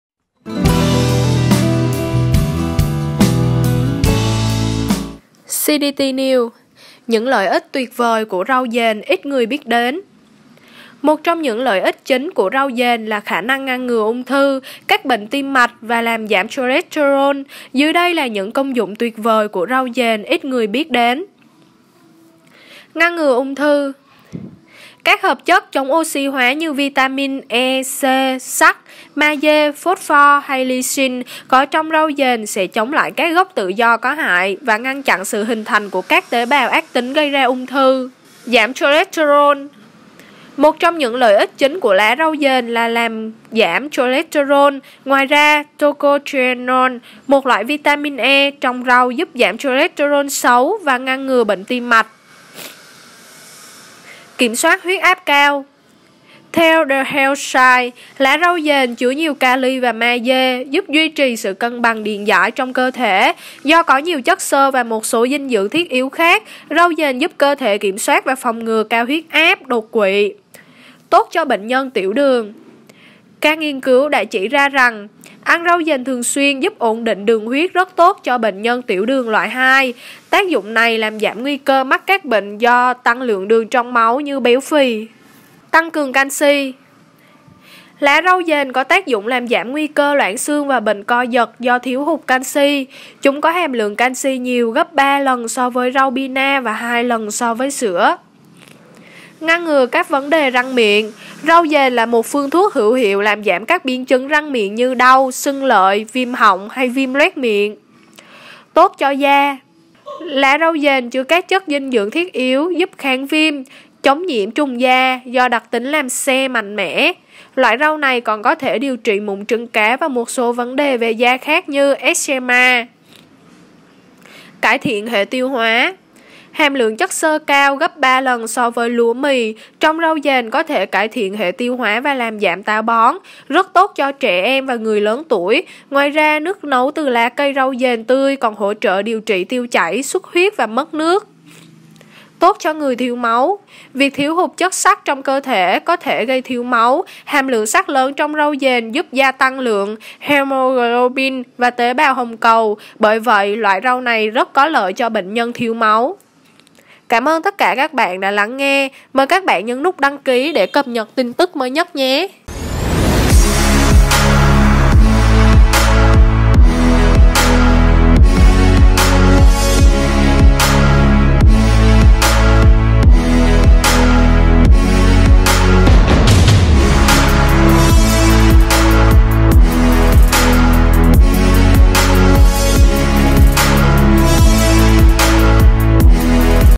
Những lợi ích tuyệt vời của rau dền ít người biết | CDT NEWS
Một trong những lợi ích chính của rau dền là khả năng ngăn ngừa ung thư, các bệnh tim mạch, và làm giảm cholesterol,...